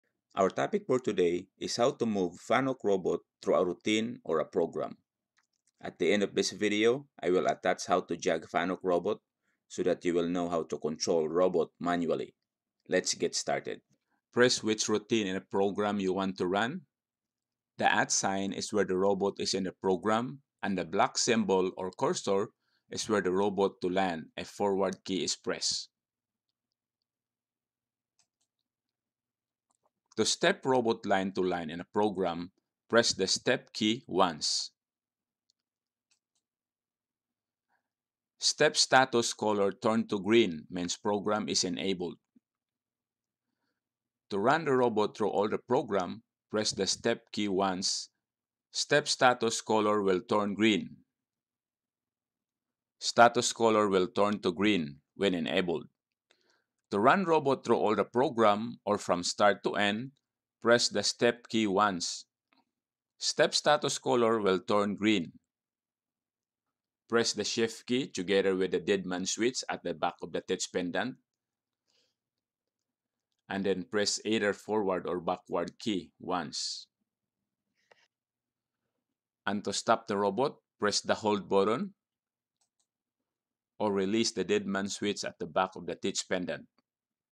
fanuc robot teach pendant training
The Teach Pendant is a hand-held robot control terminal that provides a
convenient means to move the robot, teach locations, and run robot
programs. It features a four-line 20-character LCD display, a 45-key keypad,
a live-man switch, and an emergency stop (e-stop) button.
The teach pendant is a useful tool, allowing you to move away from the host
computer terminal and control the robot locally. Typically a robot application
is programmed with teachable variables using the RAPL programming
language. Once the application is setup, it is convenient to use the teach
pendant to teach the locations for the application
In this Tutorial by Automation Academy TV which is intended to get
you started using the teach pendant. By working through the tutorial, you
become acquainted with basic pendant operation before moving on to the
details of teach pendant operation
 The teach pendant pendant consist of
• Keypad
• Live-Man Switch
• E-Stop
• Screen